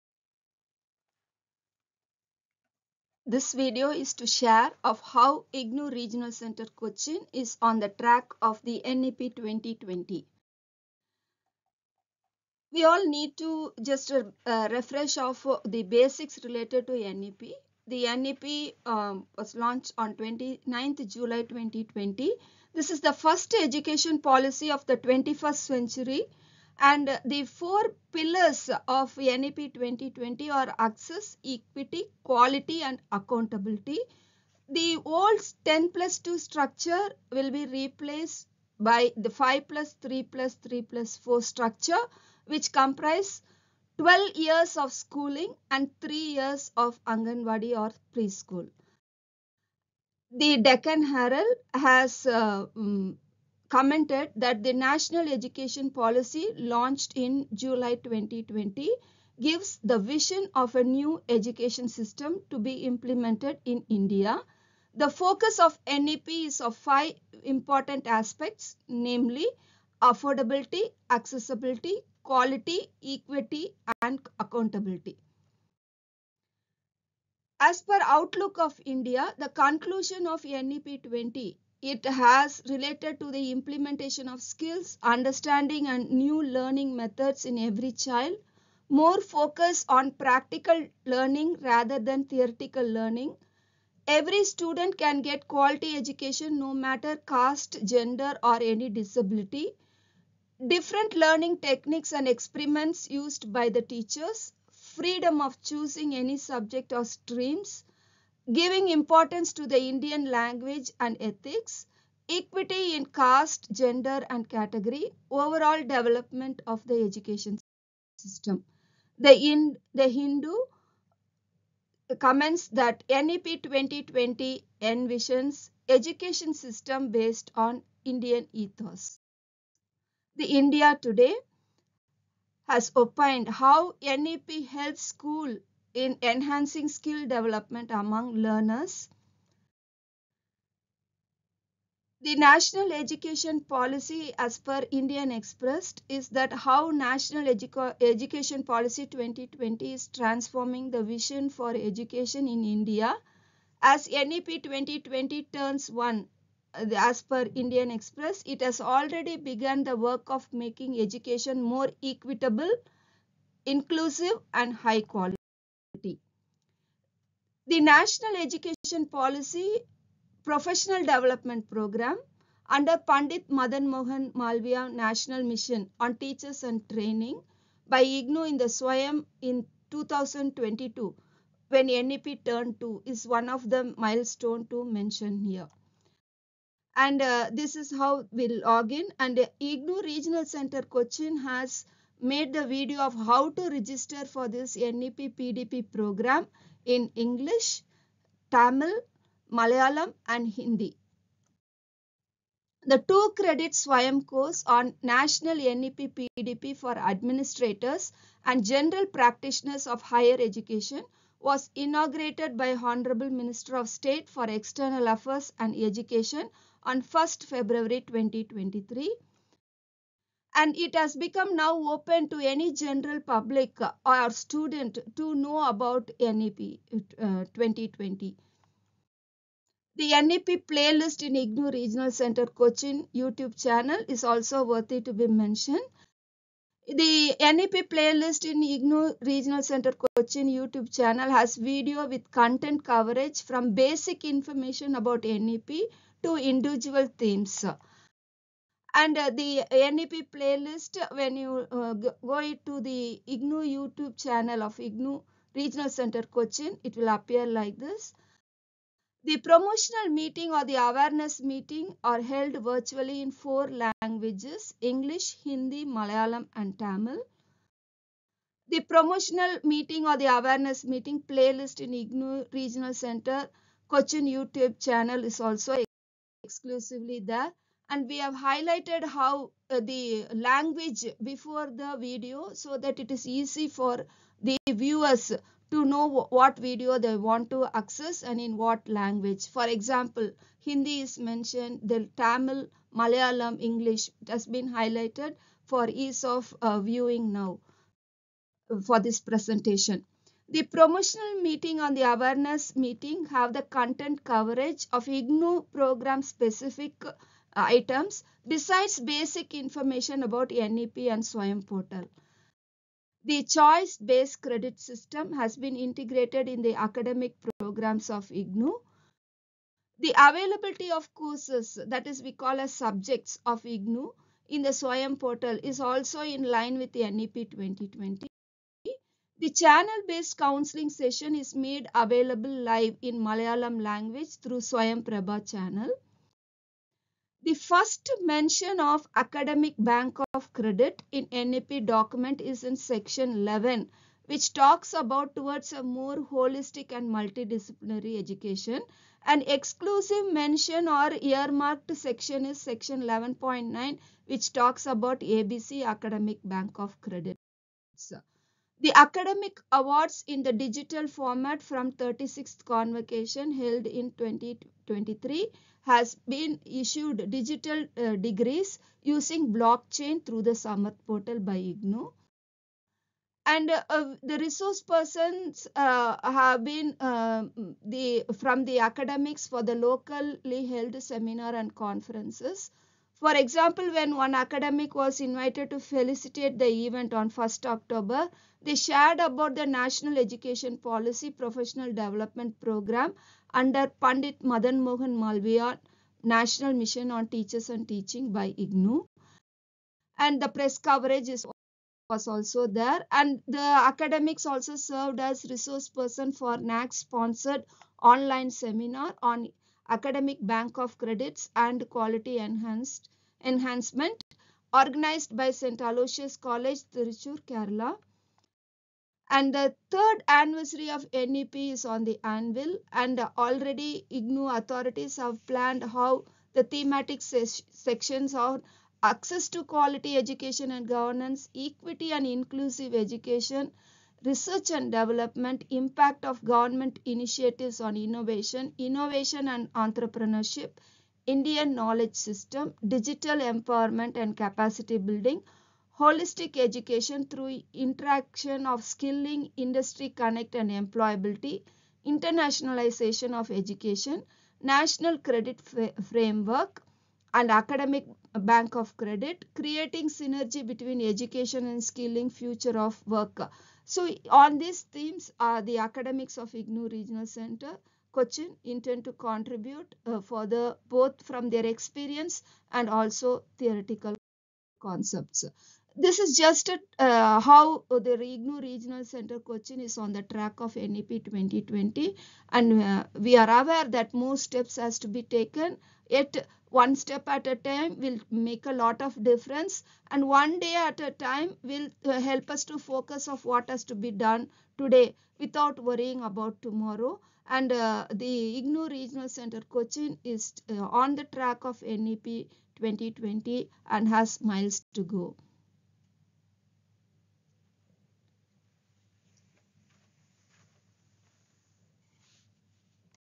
2023_English_On The Track Of NEP2020
This video in English language shares of how  the activities of IGNOU at Regional Centres are On The Track Of NEP2020.

Sh. Muhammad Ansar T.A, AE(DP) , IGNOU Regional Centre Cochin rendered Technical Expertise for preparing this video.